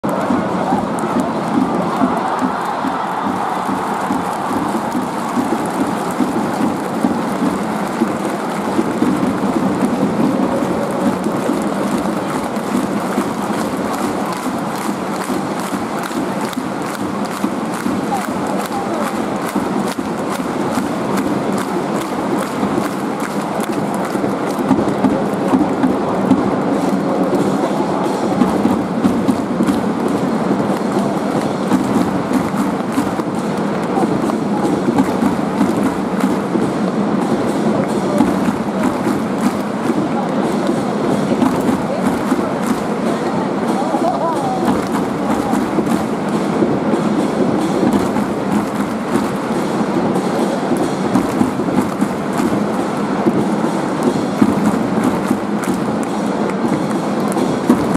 PKを蹴りたがる先輩と譲らない後輩
サッカー Jリーグ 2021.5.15 J1第14節 大分トリニータVSサガン鳥栖@昭和電工ドーム大分